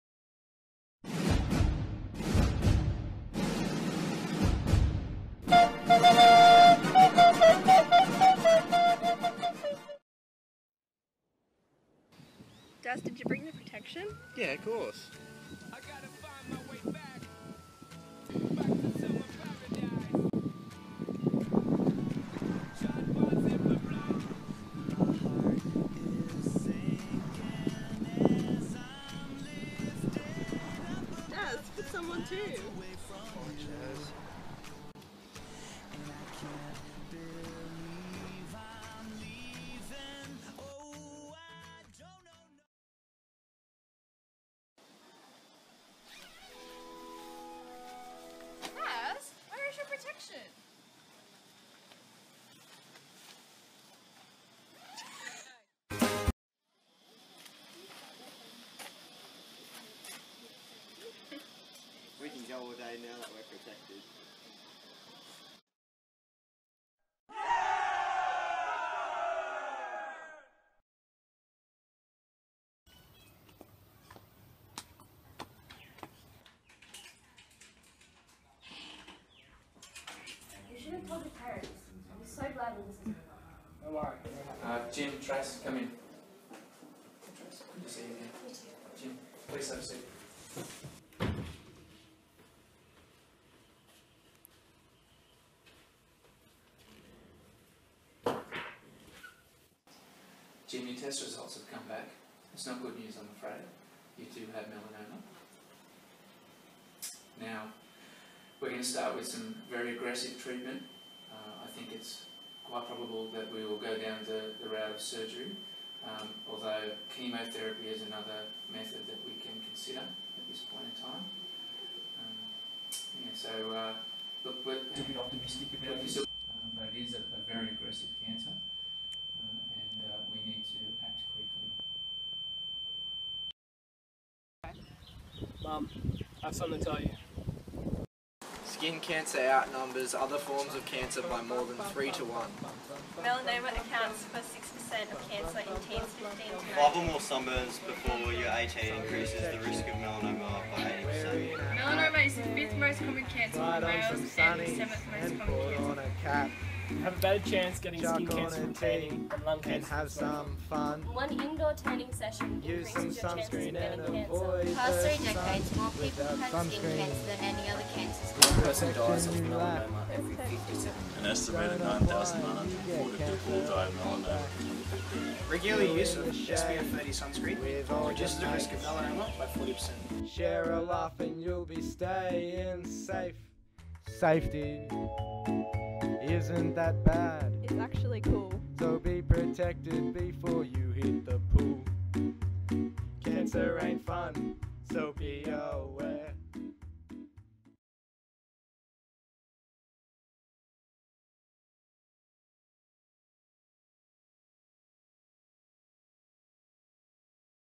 HMF and BTAC Shoot-Out: Heated Struggles of Love1
Year 10 Students at Bishop Tyrrell Anglican College raise awareness about melanoma.